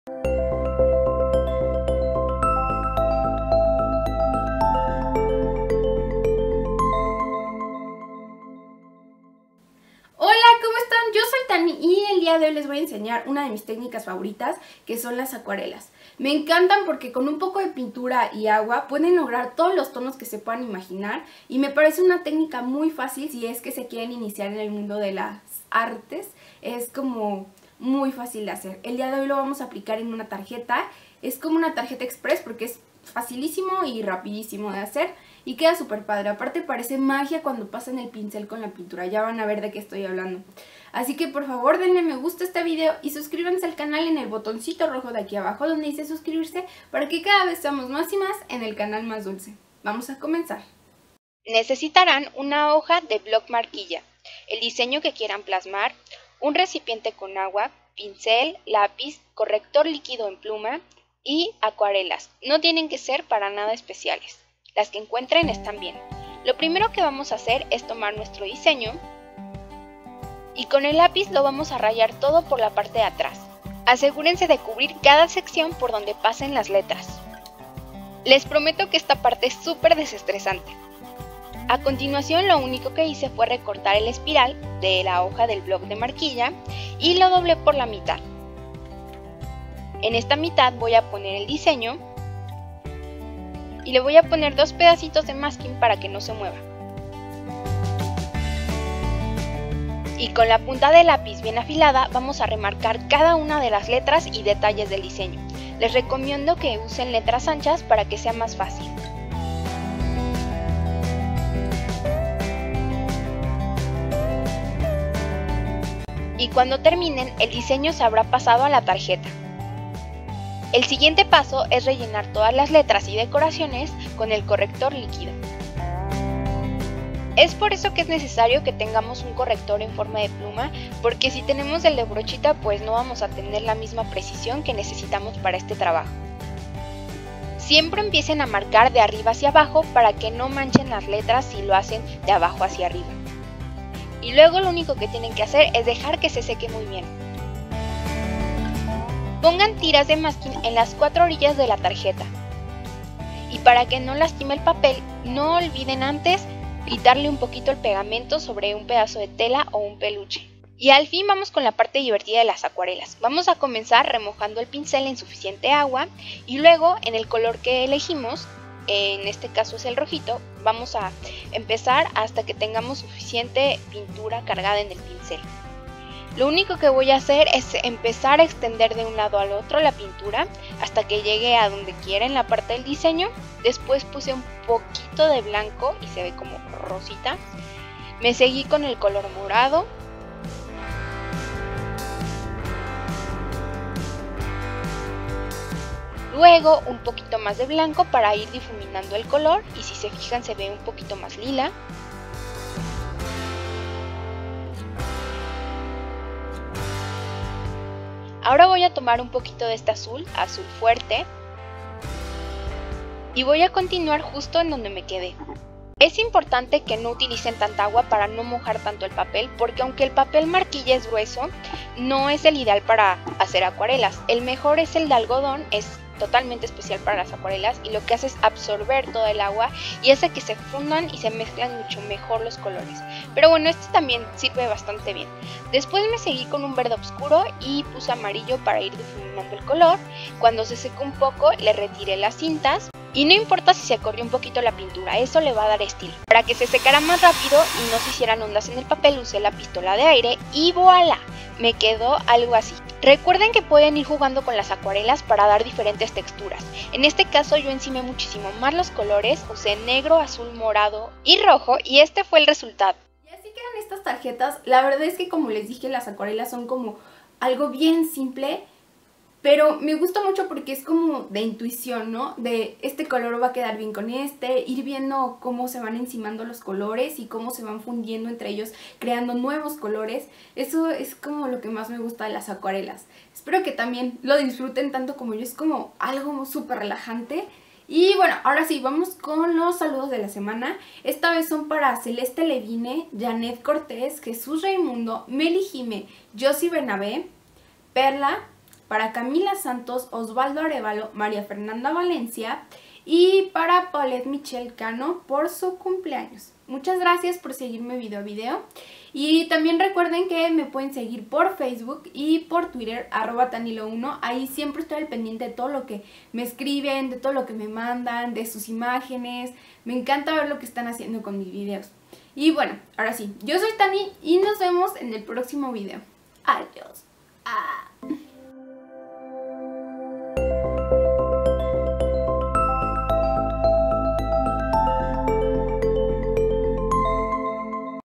Tarjetas Acuarela
No se necesita ser un experto para crear unas lindas tarjetas con acuarelas para regalar.